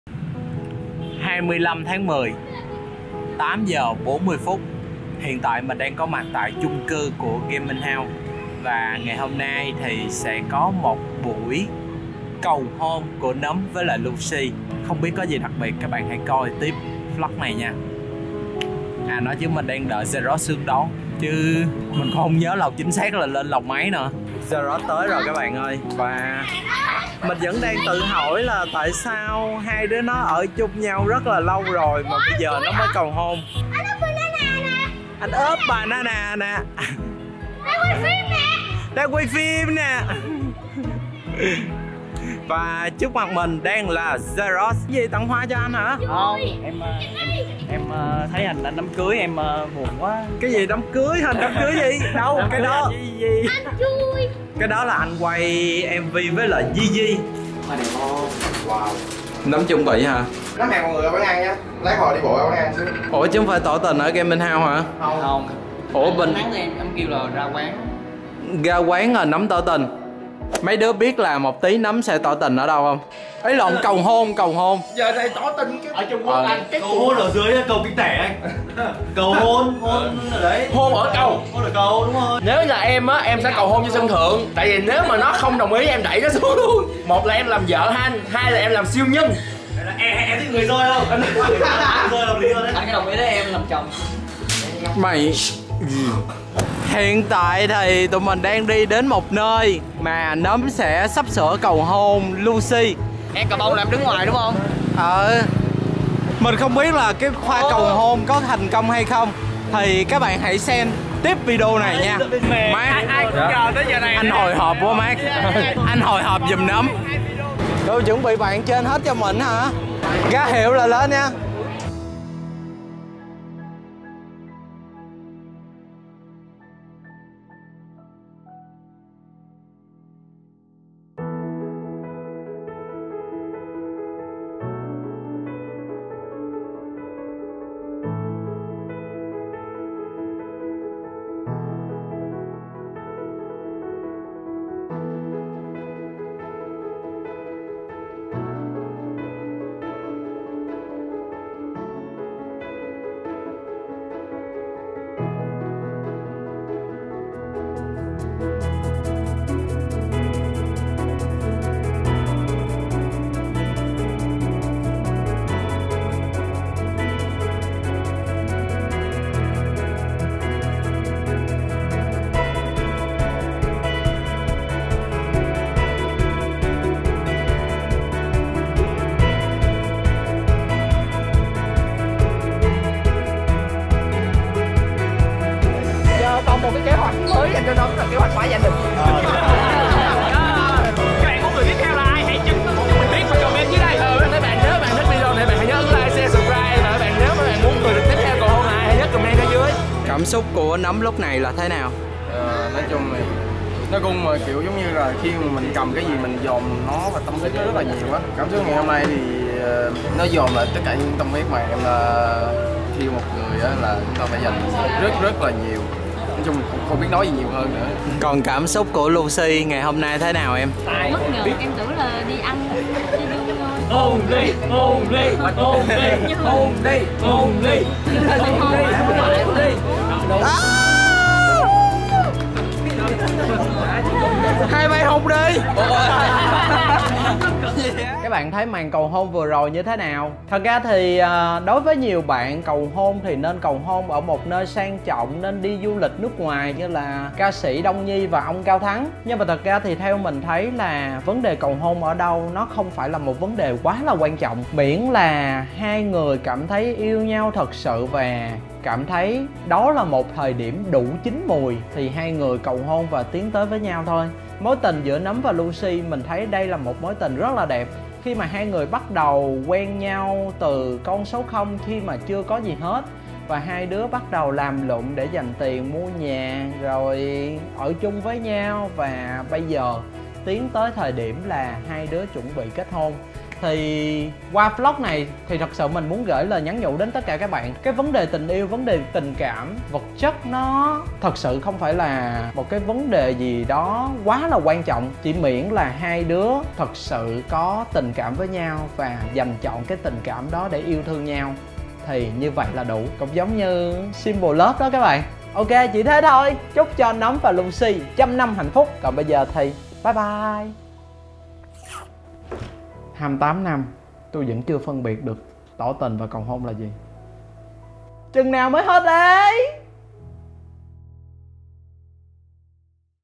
Ngày Nấm cầu hôn Lucy (Oops Banana Vlog #60)
Hôm nay là một ngày rất quan trọng trong cuộc đời của Nấm và ngày hôm nay Nấm quyết định sẽ cầu hôn Lucy.